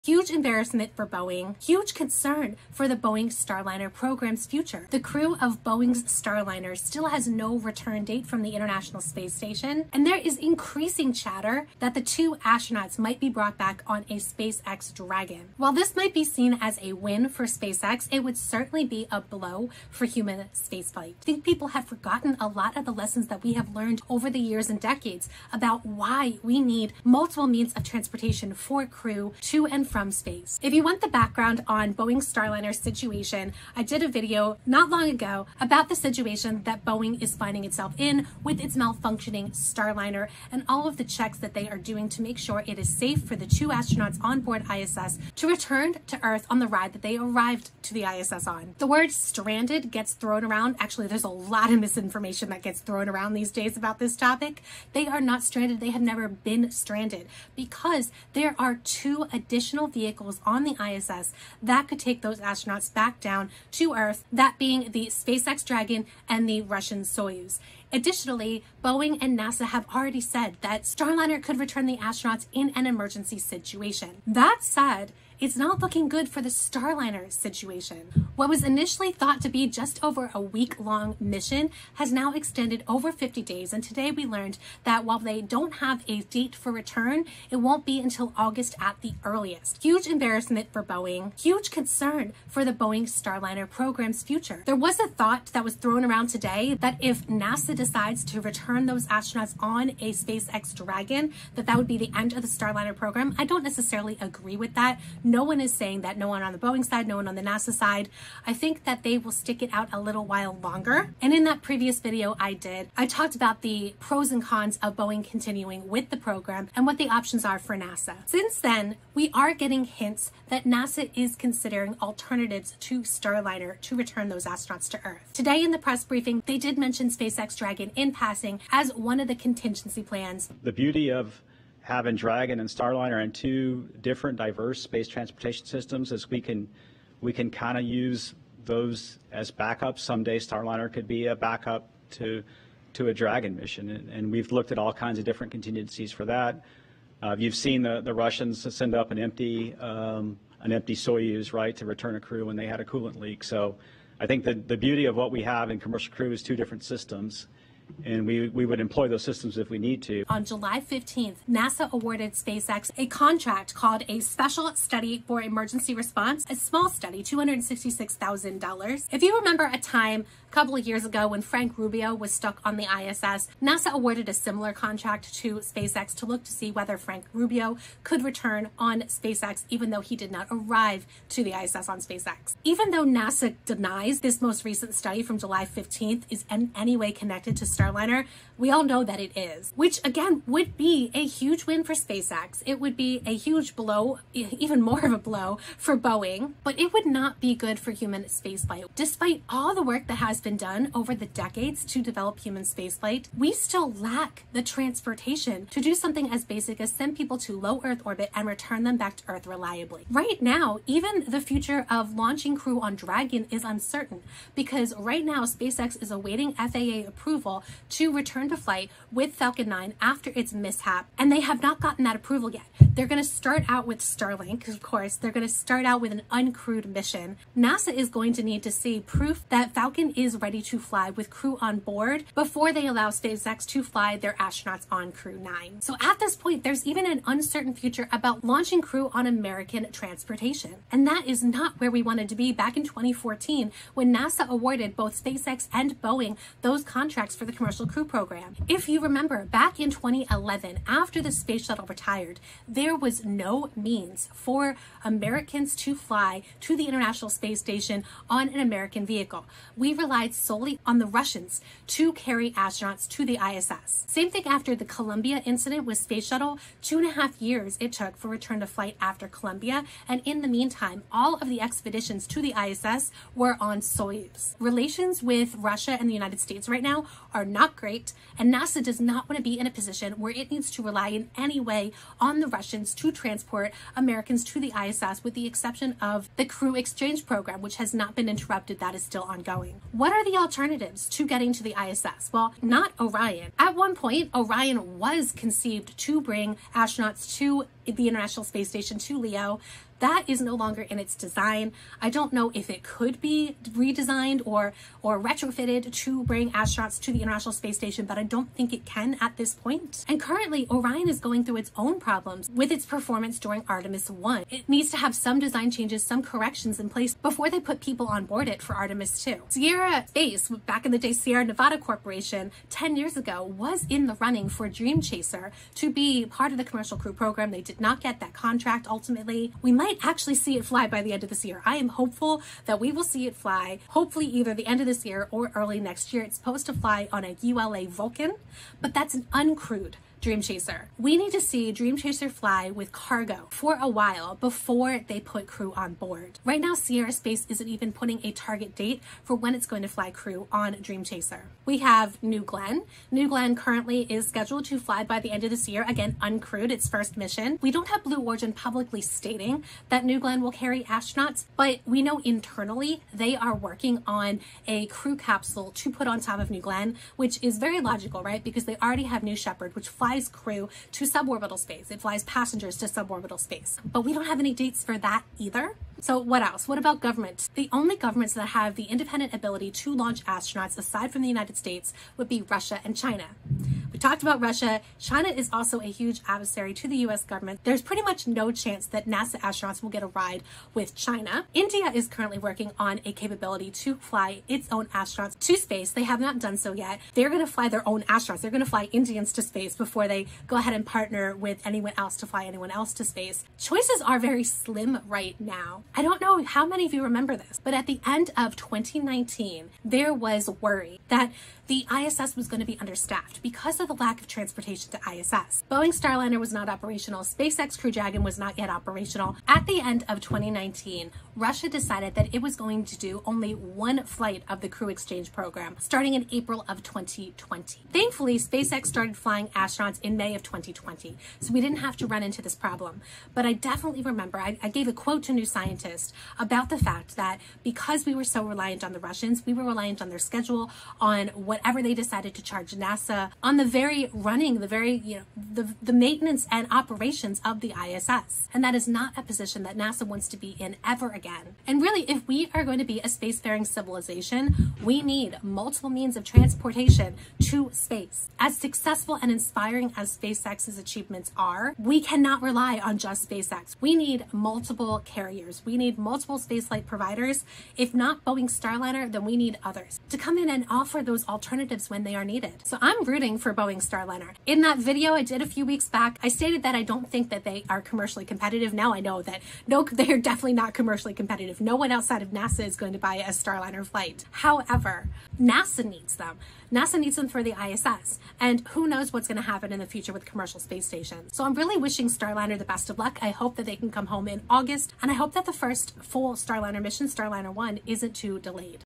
Starliner problems are a win for SpaceX, a loss for human spaceflight
With Starliner's continued problems on the ISS, Boeing loses, SpaceX wins, but ultimately human spaceflight suffers from a lack of reliable human spaceflight transportation. Multiple transportation systems are a sign of a healthy spacefaring species. Too few means of transportation can halt programs and delay progress. NASA needs multiple human spaceflight providers and dissimilar redundancy to ensure it avoids the mistakes of the past.

Note this video was created before SpaceX announced a Falcon 9 return to flight with FAA approval. Space news happens fast when SpaceX is involved!

Thumbnail background image credit: NASA

In-video image credit: Boeing, SpaceX, Sierra Space, Blue Origin

Boeing’s crewed Starliner flight won’t return until at least August, NASA says